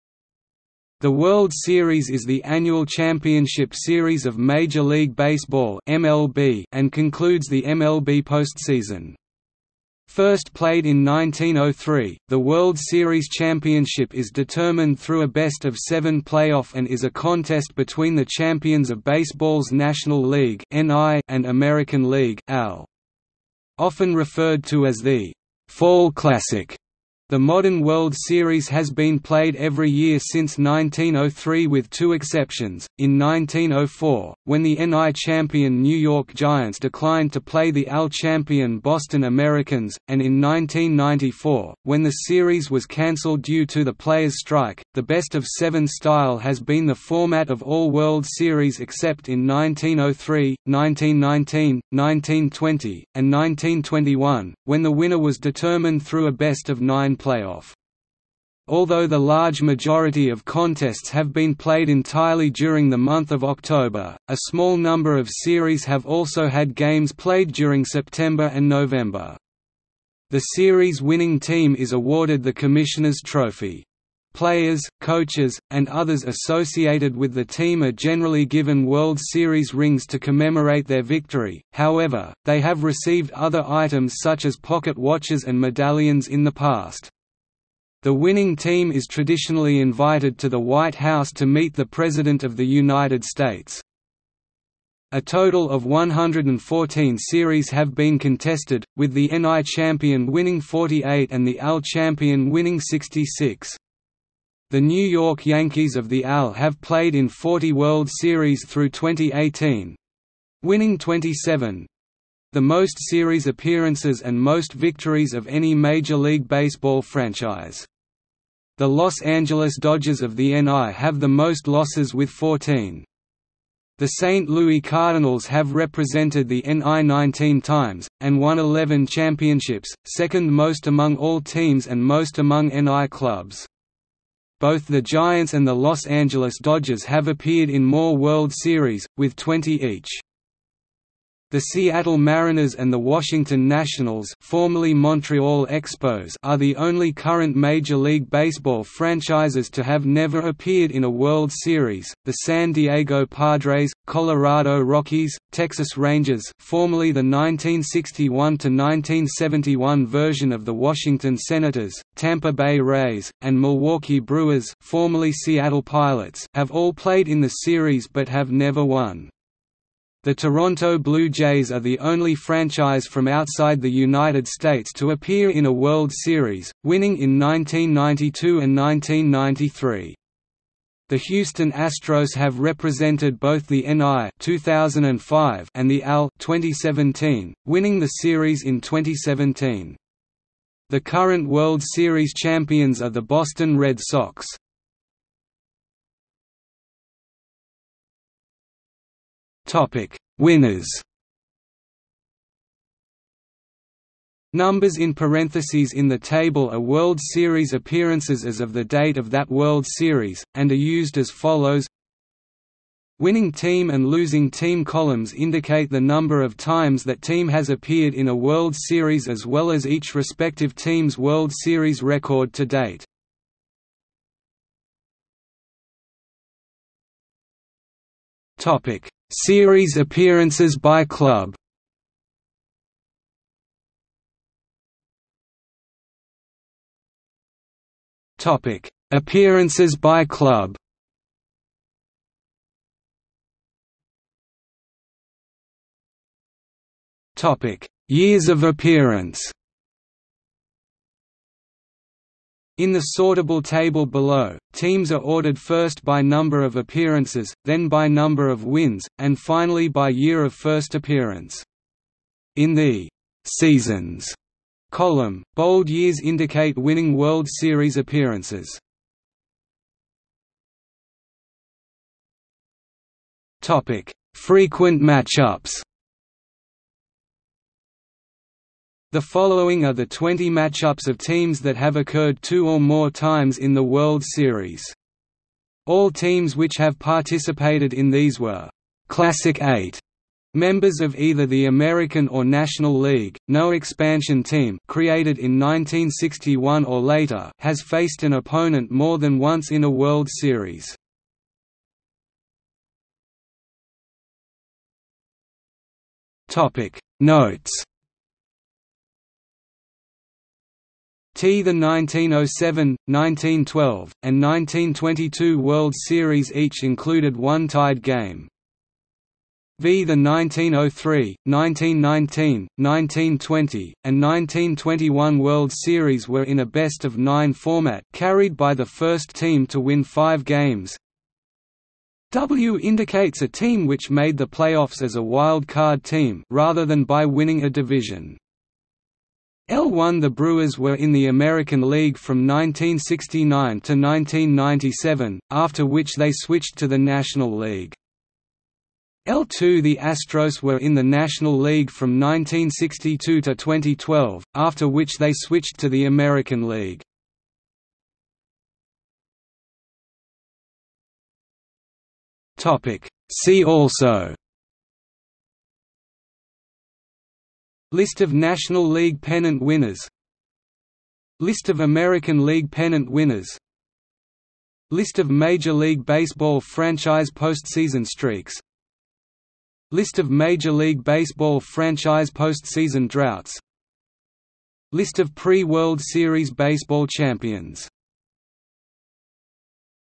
List of World Series champions | Wikipedia audio article
00:03:16 1 Winners
00:03:45 2 Series appearances by club
00:03:55 2.1 Appearances by club
00:04:04 2.2 Years of appearance
00:04:30 2.3 Frequent matchups
00:05:03 3 Notes
00:05:59 4 See also

- Socrates

SUMMARY
The World Series is the annual championship series of Major League Baseball (MLB) and concludes the MLB postseason. First played in 1903, the World Series championship is determined through a best-of-seven playoff and is a contest between the champions of baseball's National League (NL) and American League (AL). Often referred to as the "Fall Classic", the modern World Series has been played every year since 1903 with two exceptions: in 1904, when the NL champion New York Giants declined to play the AL champion Boston Americans; and in 1994, when the series was canceled due to the players' strike.The best-of-seven style has been the format of all World Series except in 1903, 1919, 1920, and 1921, when the winner was determined through a best-of-nine playoff. Although the large majority of contests have been played entirely during the month of October, a small number of Series have also had games played during September and November. The Series-winning team is awarded the Commissioner's Trophy. Players, coaches, and others associated with the team are generally given World Series rings to commemorate their victory; however, they have received other items such as pocket watches and medallions in the past. The winning team is traditionally invited to the White House to meet the President of the United States.
A total of 114 Series have been contested, with the NL champion winning 48 and the AL champion winning 66. The New York Yankees of the AL have played in 40 World Series through 2018—winning 27—the most Series appearances and most victories of any Major League Baseball franchise. The Los Angeles Dodgers of the NL have the most losses with 14. The St. Louis Cardinals have represented the NL 19 times, and won 11 championships, second-most among all teams and most among NL clubs. Both the Giants and the Los Angeles Dodgers have appeared in more World Series, with 20 each.
The Seattle Mariners and the Washington Nationals (formerly Montreal Expos) are the only current Major League Baseball franchises to have never appeared in a World Series; the San Diego Padres, Colorado Rockies, Texas Rangers (formerly the 1961–1971 version of the Washington Senators), Tampa Bay Rays, and Milwaukee Brewers (formerly Seattle Pilots) have all played in the Series but have never won. The Toronto Blue Jays are the only franchise from outside the United States to appear in a World Series, winning in 1992 and 1993. The Houston Astros have represented both the NL (2005) and the AL (2017), winning the Series in 2017. The current World Series champions are the Boston Red Sox.